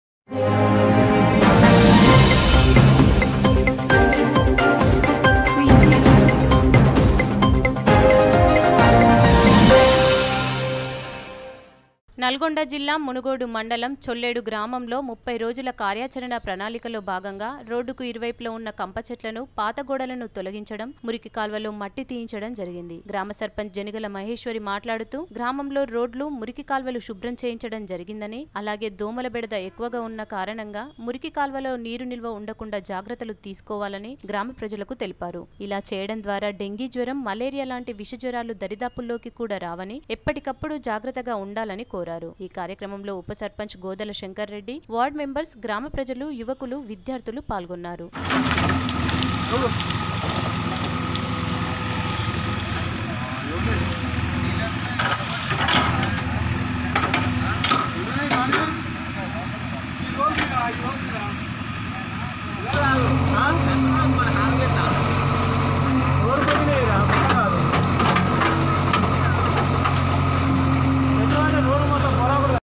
చొల్లేడు గ్రామంలో రోడ్లు, మురికి కాల్వలు శుభ్రం || NEWS 9 TV
నల్గొండ జిల్లా మునుగోడు మండలం చొల్లేడు గ్రామంలో 30 రోజుల కార్యాచరణ ప్రణాళికలో భాగంగా రోడ్డుకు ఇరువైపుల వున్న కంపచెట్లను, పాతగోడలను  తొలిగించటం, మురికి కాల్వలలో మట్టి తీయించటం జరిగింది. గ్రామ సర్పంచ్ జనిగల మహేశ్వరి మాట్లాడుతూ గ్రామంలో రోడ్లు, మురికి కాల్వలు శుభ్రం చేయించటం జరిగిందని, అలాగే దోమల బెడద ఎక్కువగా వున్న కారణంగా మురికి కాల్వలలో నీరు నిల్వ ఉండకుండా జాగ్రత్తలు తీసుకోవాలని, గ్రామ ప్రజలకు తెలిపారు. ఇలా చేయడం ద్వారా డెంగీ జ్వరం, మలేరియా లాంటి విషజ్వరాలు దరిదాపుల్లోకి కూడా రావని, ఎప్పటికప్పుడు జాగ్రత్తగా ఉండాలని కోరారు. ఈ కార్యక్రమంలో ఉపసర్పంచ్ గోదల శంకర్ రెడ్డి, వార్డ్ మెంబెర్స్, గ్రామప్రజలు, యువకులు, విద్యార్థులు పాల్గొన్నారు.